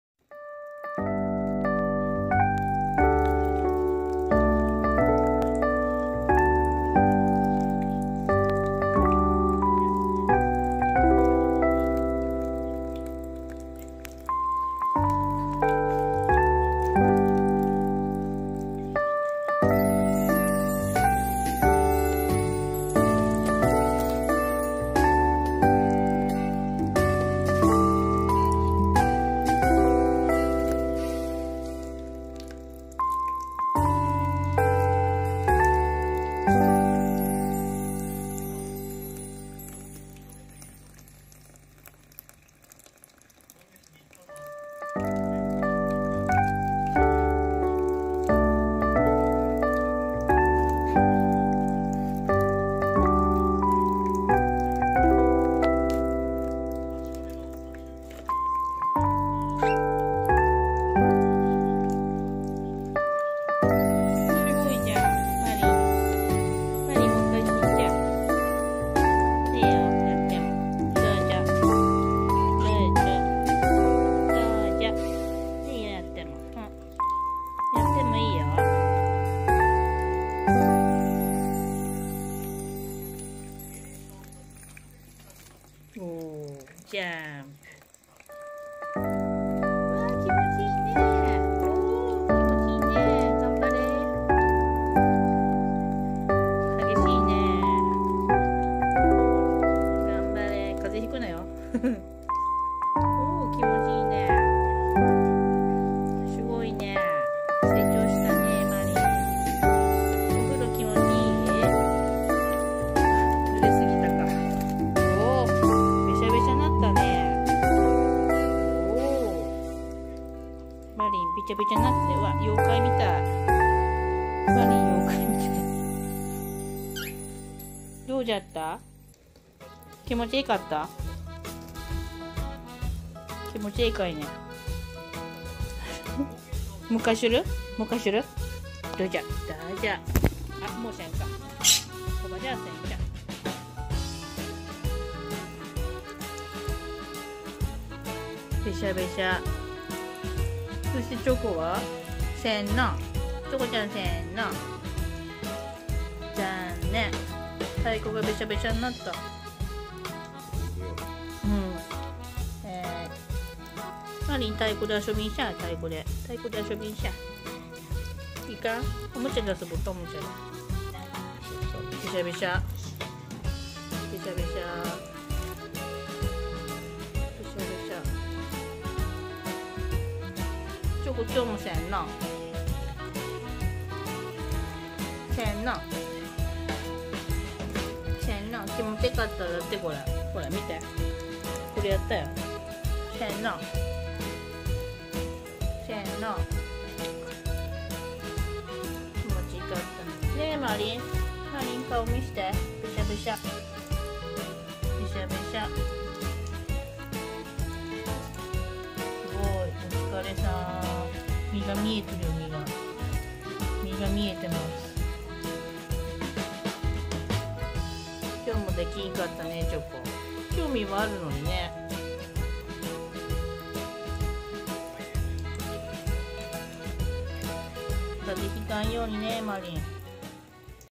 今日のチョコ&マリン😆💕🐥🎶(オキナインコ&オカメインコ)
JUNママお誕生日で、チョコ&マリンと相方メロン🍈ちゃんからコメント入りです😆🎵🎵